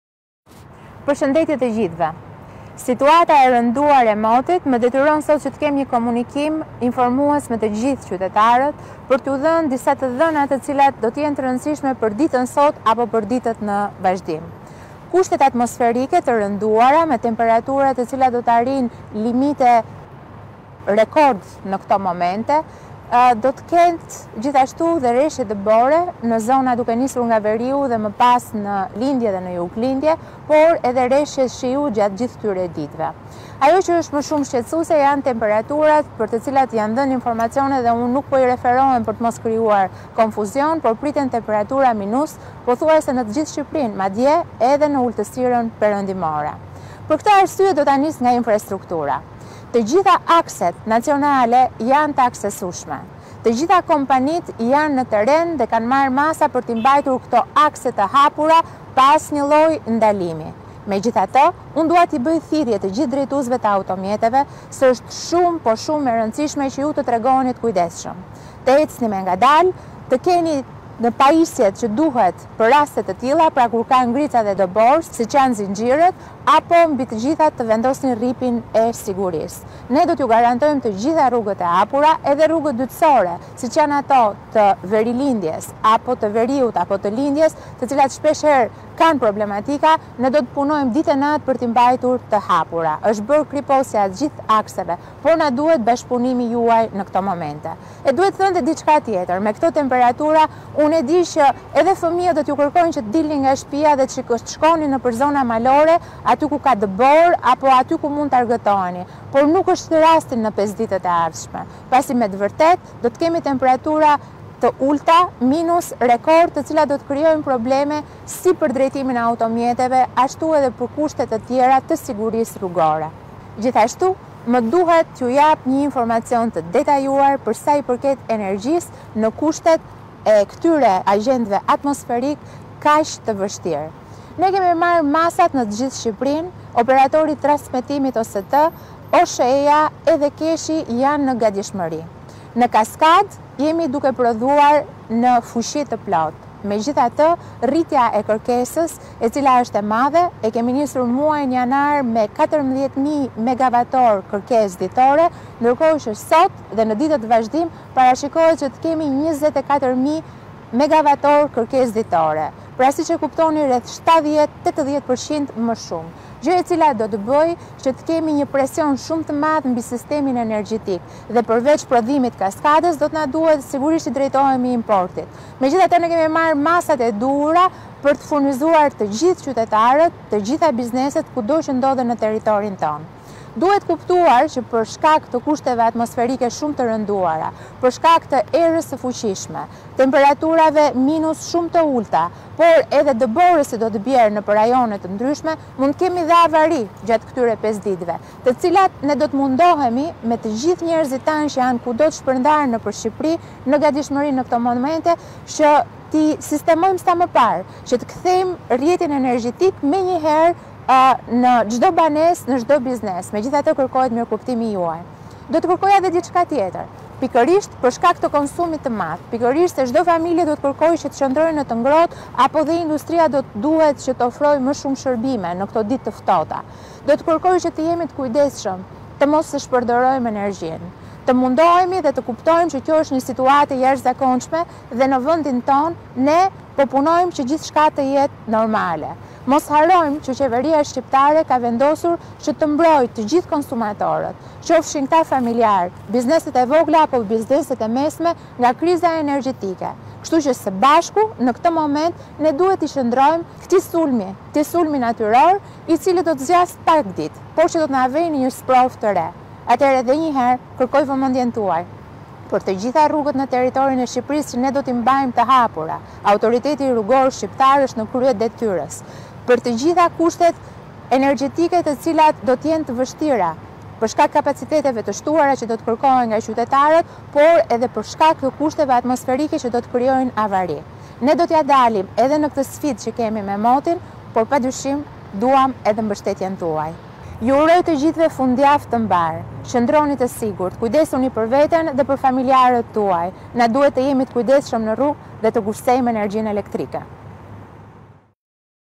Balluku-qytetarëve: Të gjitha akset do të jenë të kalueshme!5 ditët e ardhshme mos dilni për turizëm
5 ditët e ardhshme nëse keni ndërmend të shkoni në ndonjë udhëtim në ndonjë vend me borë për ministren e Infrastrukturës Belinda Balluku do të duhet ta rimendoni. Në një apel qytetarëve Balluku thotë se temperaturat do të jenë ekstreme.